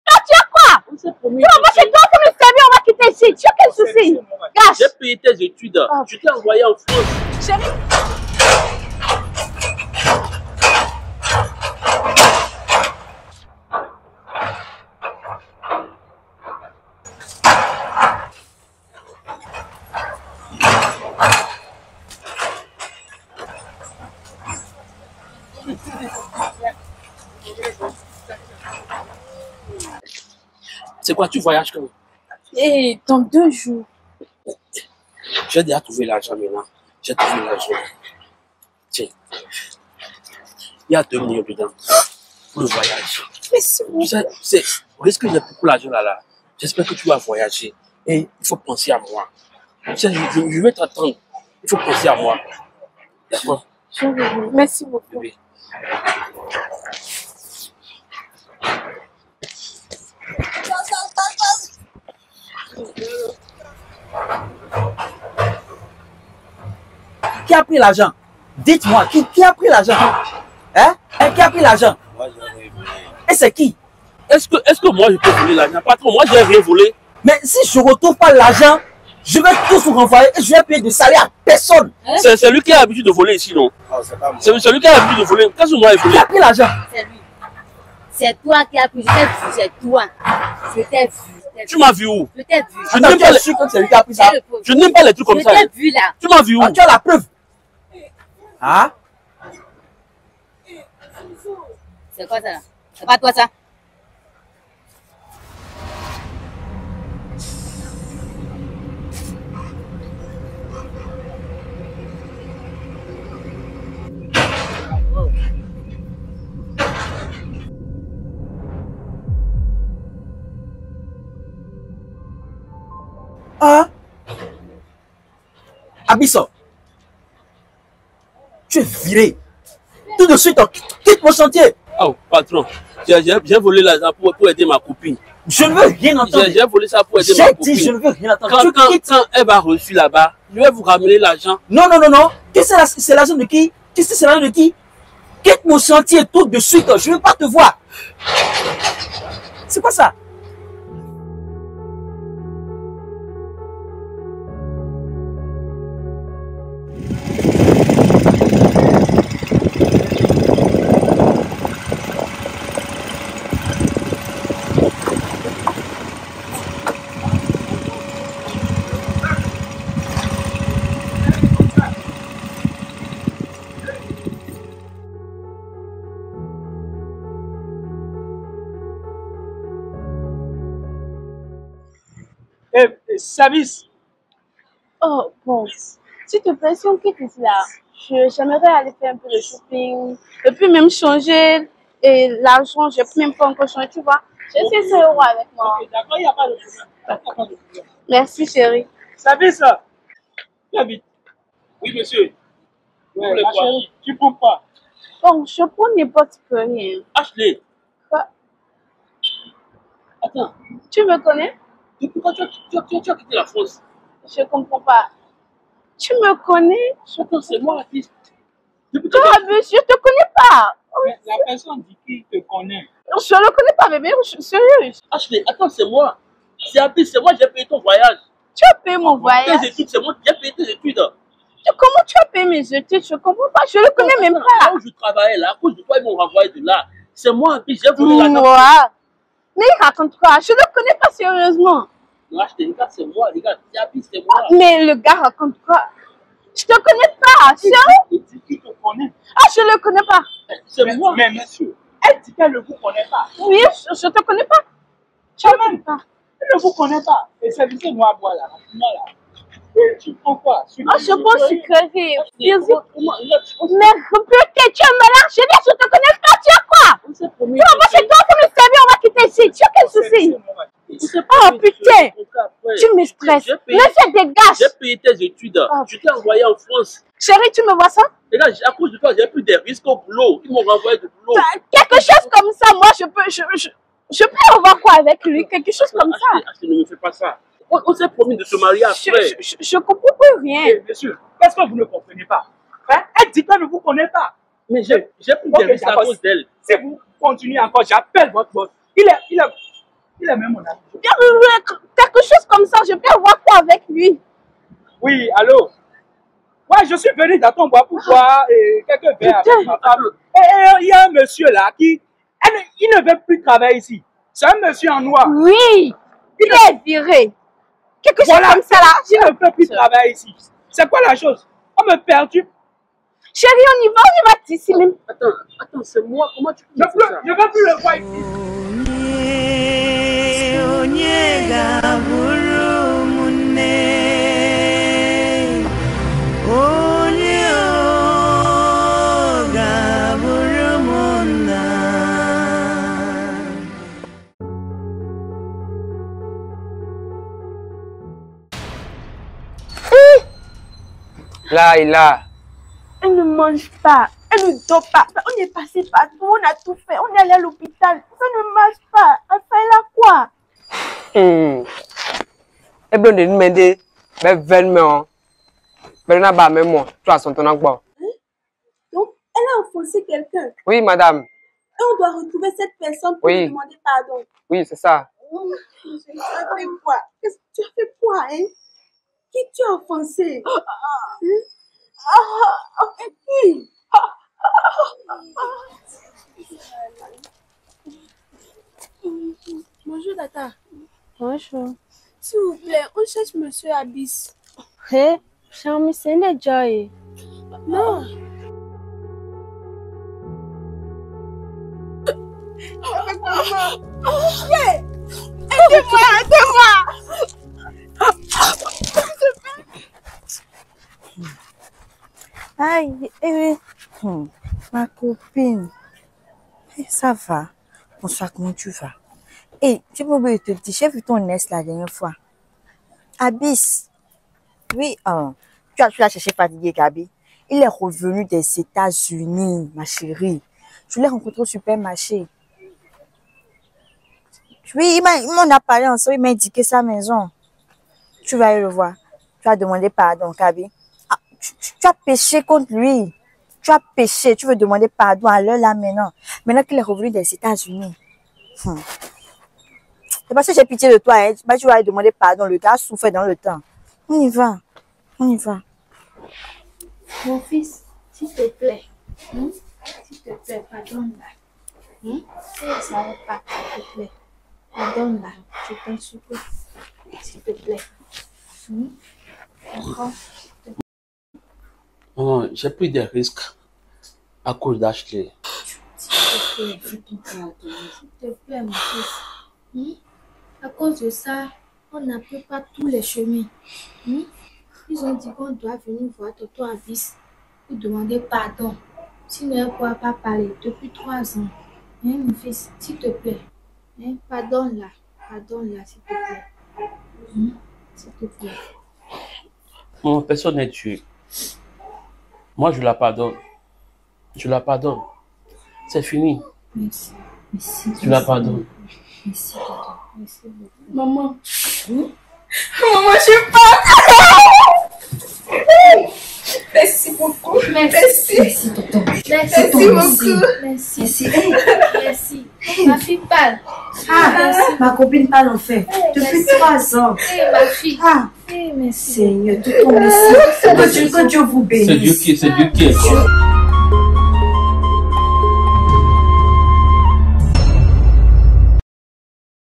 ELLE M'A TRAHI ET ÇA A MAL FINI POUR ELLE.
Ma copine rêvait de voyager et d'étudier en France, mais elle n'avait pas assez d'argent. Par amour et pour l'aider à réaliser ses rêves, j'ai pris une décision qui allait tout changer : voler de l'argent à mon patron pour financer son voyage.

Je savais que c'était risqué, mais je pensais que l'amour que nous partagions valait tous les sacrifices. Mais après, tout a mal tourné. Mon patron a découvert le vol, j'ai perdu mon travail, et ma vie s'est effondrée.

Le pire est arrivé lorsque ma copine est rentrée de son voyage. Au lieu de me remercier ou de m'aider à reconstruire ma vie, elle a agi comme si elle ne me connaissait pas. Elle m'a trahi, et j'ai tout perdu pour elle.